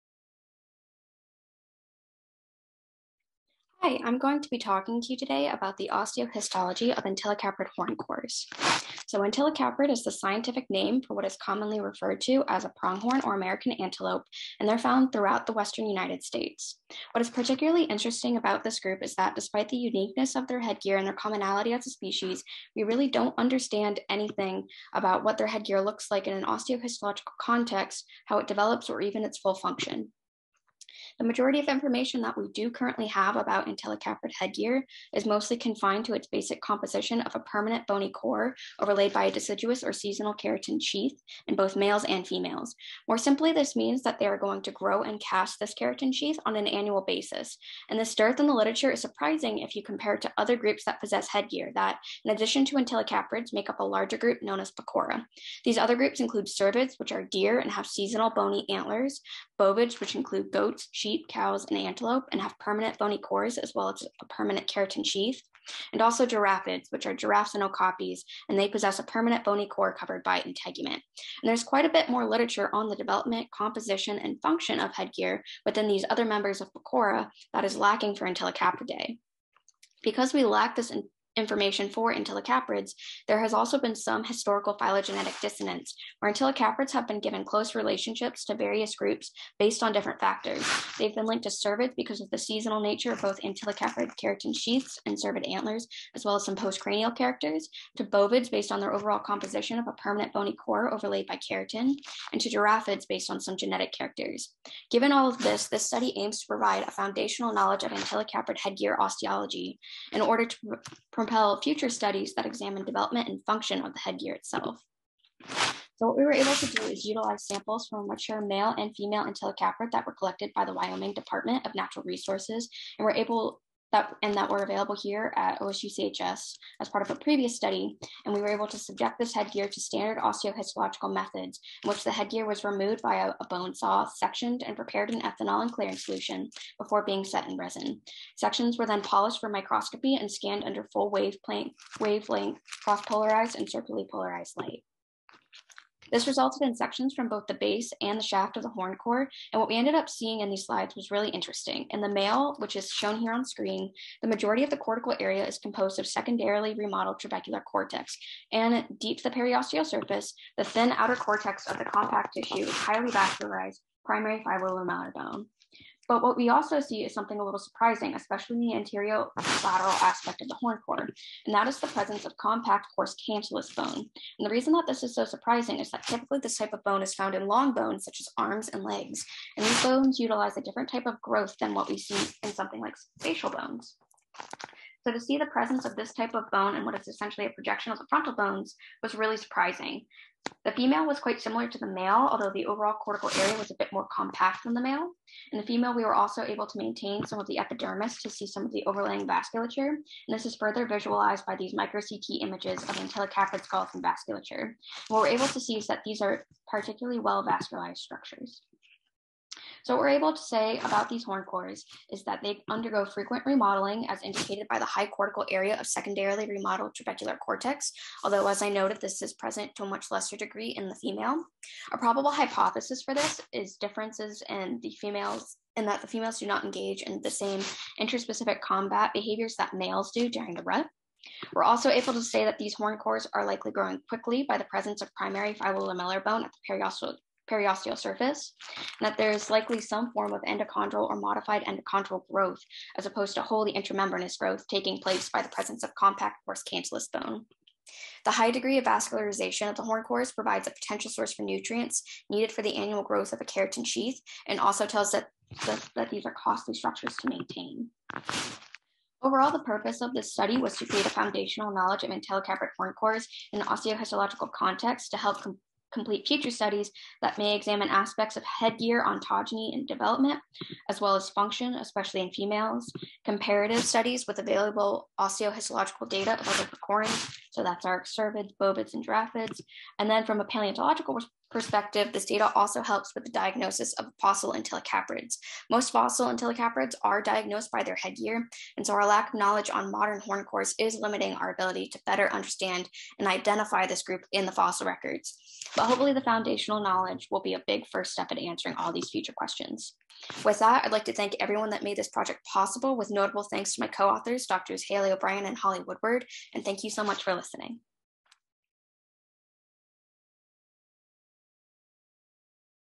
Research Week 2022: Slenker
Osteohistology of Antilocaprid Horncores, Katherine Slenker, BA, Holly Woodward, Ph.D., Haley O'Brien, Ph.D. Katherine